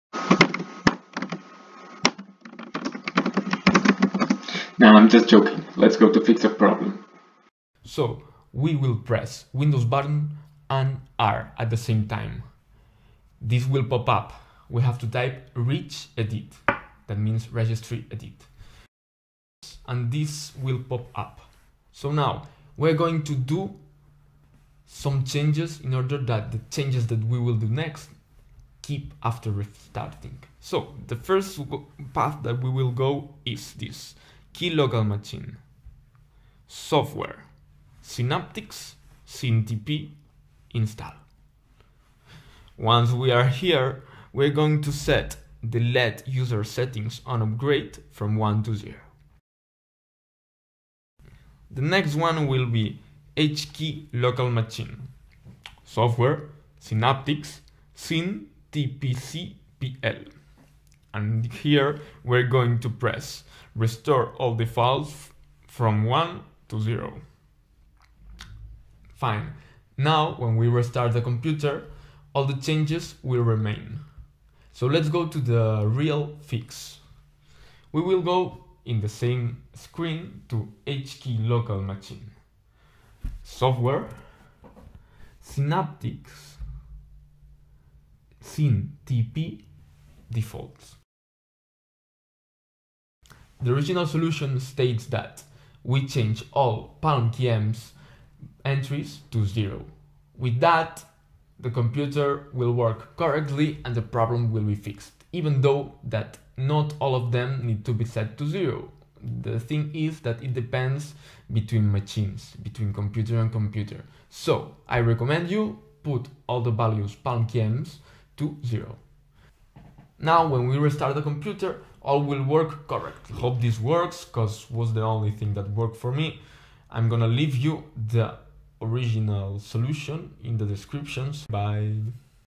Can't use the keyboard and touchpad at the same time (Disable Smartsense on Synaptics Touchpad)
Didn't work? Try this new solution for laptops using Windows 11 and 10.

Note that this solution will only work with older laptops using Synaptics probably using Windows 7, 8 or 10. If you can't find the PalmKms parameters means you don't have Synaptics and can't use this solution.

Easy and quick solution to fix the problem the problem: can't move the laptop touchpad cursor mouse at the same time while pressing the keyboard and typing. Solved after multiple attempts!